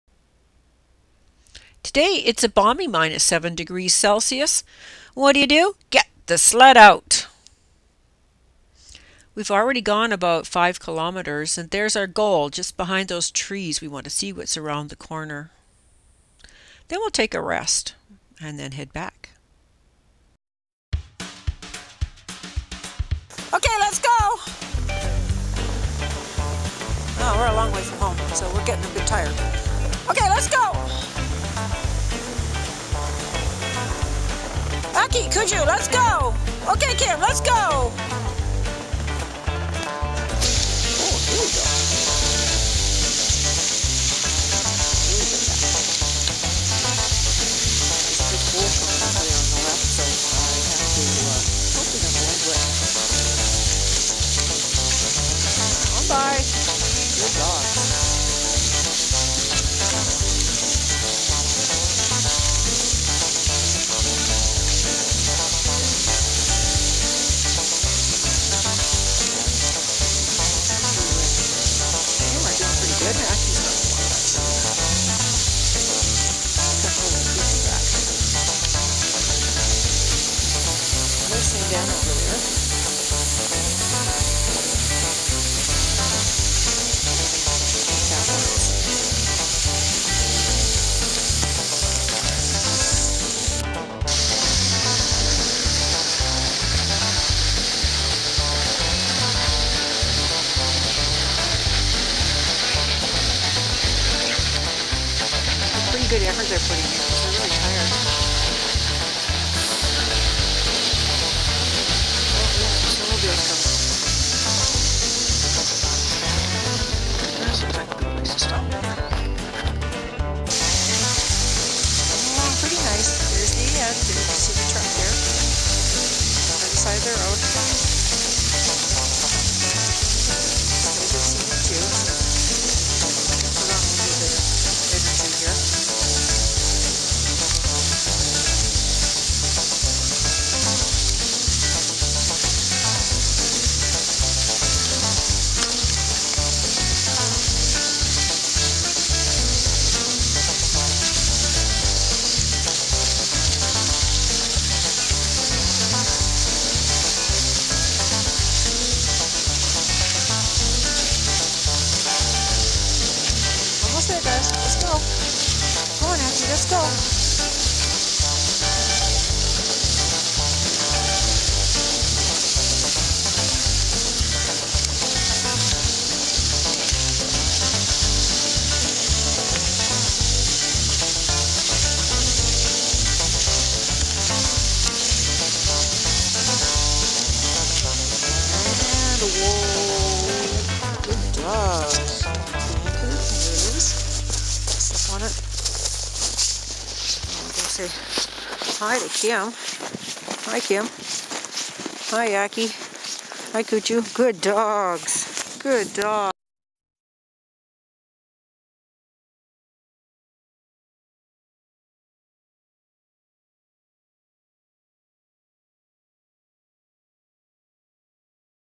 Get The Sled Out
Kathy takes the huskies out for a 10 km sled run through snowmobile trails in Eastern Ontario.